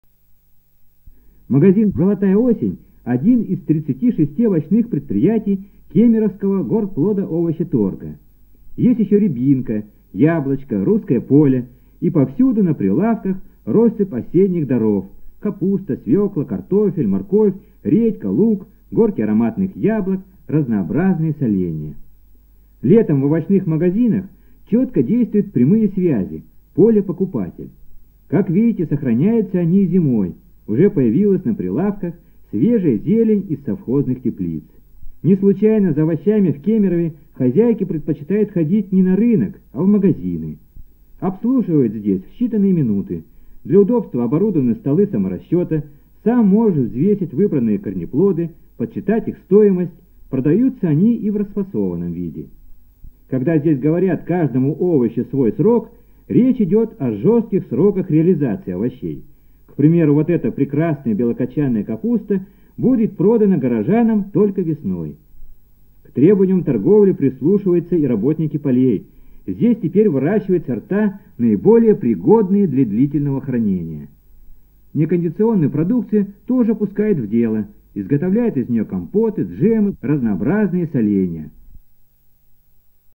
Дары осени - ранней весной. Новости. Эфир 5 марта 1980
Сюжет о работе овощного магазина "Золотая осень" одного из предприятий Горплодовощеторга в Кемеровской области.

Информационный сюжет "Щедра осень в зимний день". Эфир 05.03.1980.